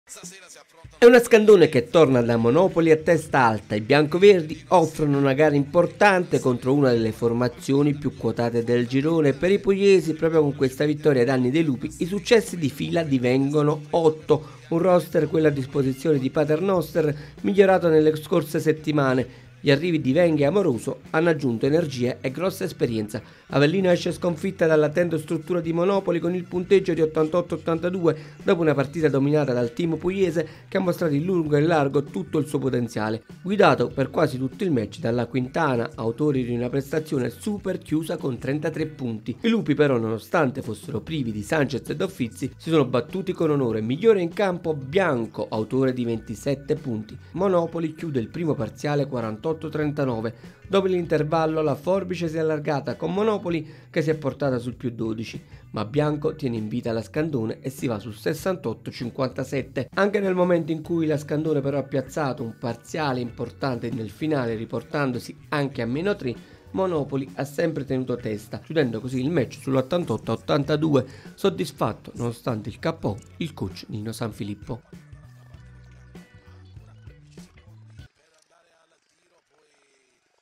Scandone a testa altra contro Monopoli: 88-82 il finale
I biancoverdi vengono sconfitti, ma disputano una grande prestazione contro una delle squadre piu' quotate.